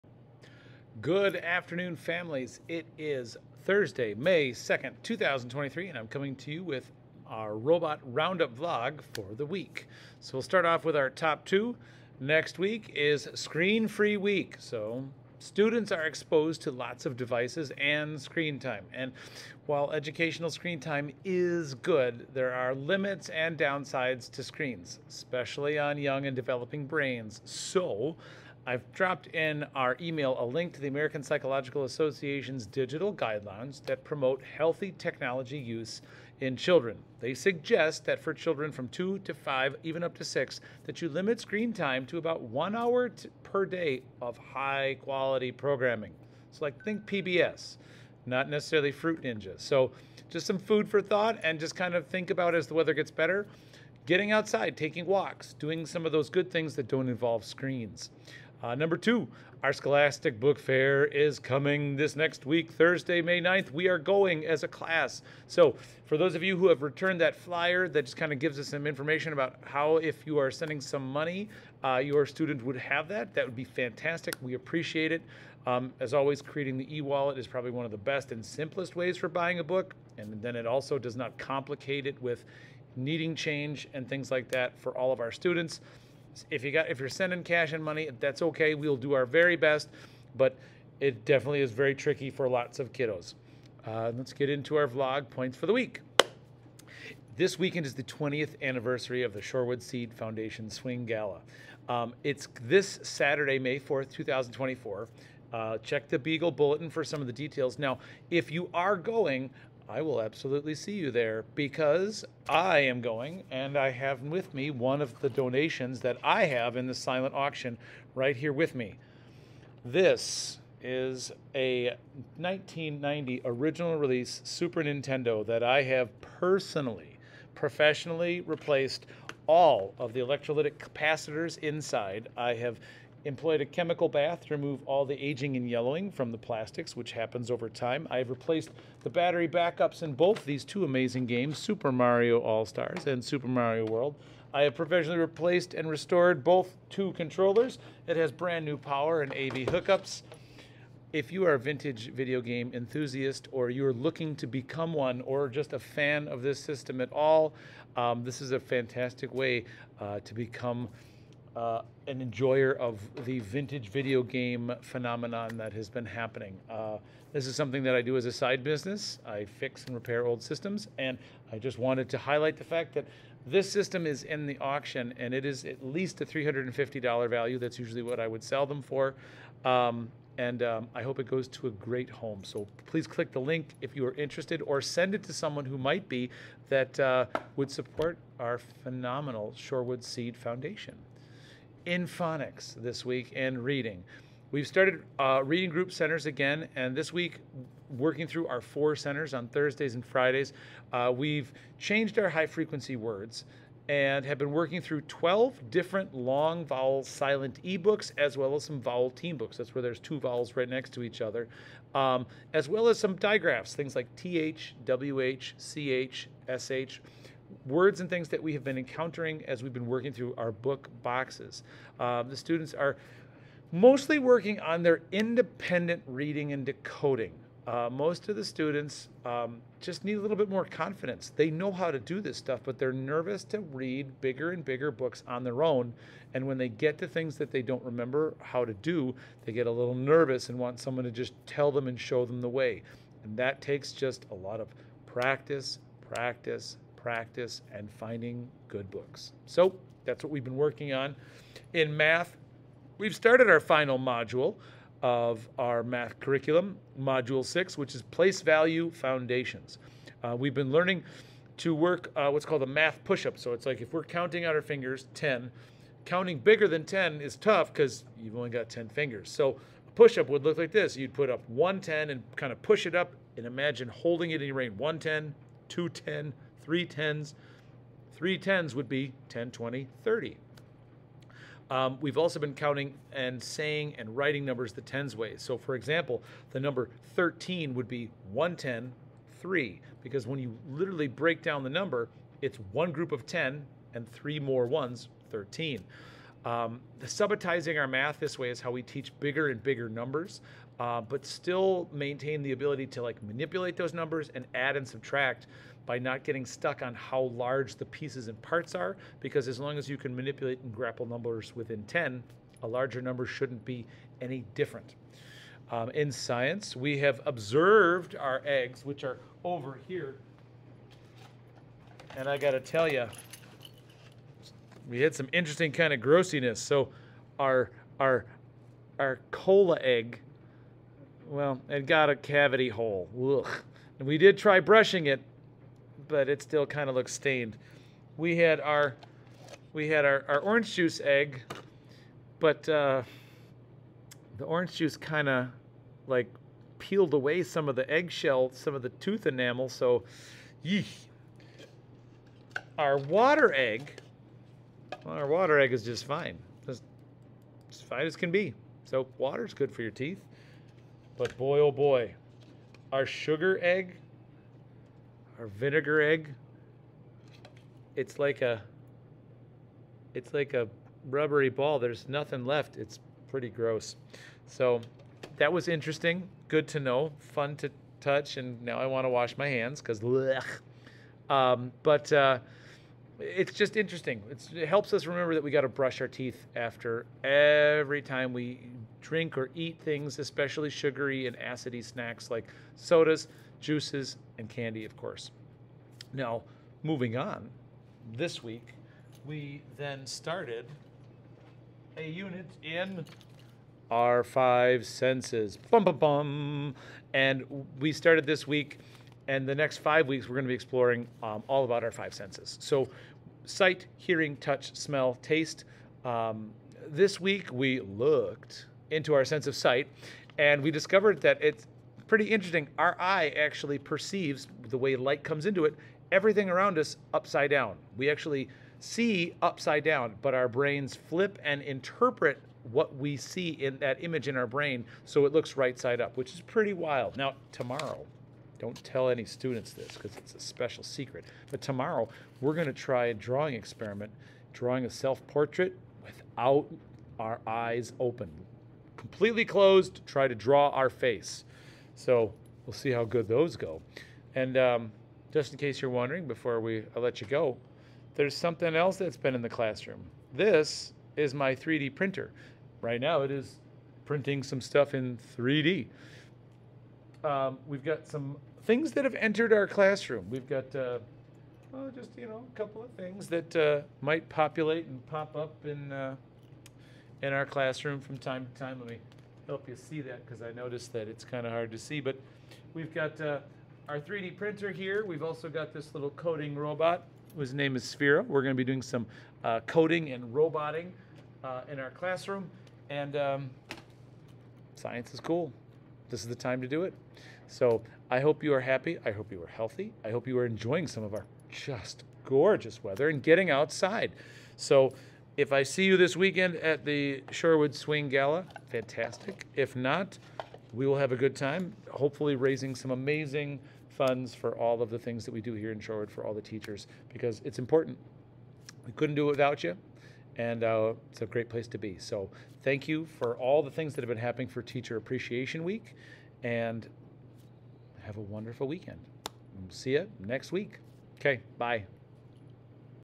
Mr Dittl's Robots Roundup Vlog for the week of 05 02 2024
*               This Week’s Top two are…                     *
• Next week is a screen-free week! Students are exposed to lots of devices and screen time. While educational screen time is good, there are also limits and downsides to screens; especially on young developing brains. Here’s a link to the American Psychological  Association digital guidelines promoting healthy technology use in children. They suggest, “For children 2 to 5, limit screen time to one hour per day of high-quality programming.”

• The scholastic book fair is on Thursday, May 9th! Please return the flyer that came home in last Thursday's folder. Returning this helps us support your student when we go as a class. Creating an e-wallet for your student is a great way to simplify buying a book and not complicate the process with physical change. Sending money is also ok, but 20 bags of money for 5 & 6-year-olds can be tricky to manage!
**Vlog points for the week include:
The 20th Anniversary Shorewood SEED Foundation Swing Gala is this Saturday, May 4th, 2024! If you are going, I will definitely see you there. You can click the link here to see one of my donations to the auction: an original 1990 Super Nintendo I personally restored to showroom condition! I professionally replaced all of the electrolytic capacitors, employed a chemical and UV light bath to remove all yellowing in the plastic, and replaced the battery save-back-ups on two original games (Super Mario Word & Super Mario All-Stars, making this a beautiful vintage system to own and enjoy! If you’re a retro-game enthusiast or looking to start a vintage system collection, look no further.

•In phonics & reading groups, we started our reading group centers again this week. Our class works through 4 stations during the day on Thursday and Friday. This week we changed our high-frequency words and worked through 12 different long vowel-silent -e books as well as some vowel teams and di-graphs. The students are working more on their independent confidence than anything else.

• In math, we started the final Module of our math curriculum, Module 6: Place Value Foundations. We learned to work a math push-up which is counting number bigger than 10 using a physical hand strategy. We also began counting, saying, and writing numbers “the 10’s way.” For example, the number 13  is called one-10 three which means it is a group of 1 set of ten and three extras. This subatizing in math is how we teach bigger and bigger numbers and still manipulate, add, and subtract them.
In Science, we observed our “egg” teeth and found that the soda egg had gotten a cavity hole! The orange juice egg had the tooth enamel peeling and flaking off. The vinegar egg has completely lost its shell/enamel and feels like a rubbery balloon. The water egg tooth, however, was just fine and seemed to suffer no negative effects. the effects of not brushing teeth after different drinks.
On Wednesday, we began our Five Senses science exploration. Over the next five weeks, we will explore and learn about our five senses; sight, hearing, touch, smell, and taste. This week we “looked” into our sense of sight and discovered that our eyes actually perceive the world around us upside-down. Our brains interpret that image and flip it right-side-up! Pretty wild! Tomorrow we will try an experiment drawing a self-portrait without looking!
We have a 3D printer in our room as well as a Sphero + coding robot. Science technology, engineering, and math are amazing and I love bringing those things into the classroom to inspire students to be curious about how they can change the world for the better!